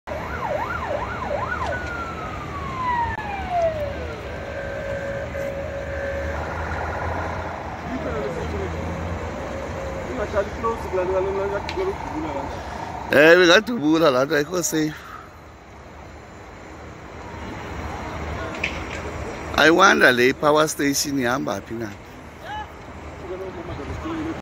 Komani substaion on fire
The Western substation in Komani caught fire on May 6, 2022.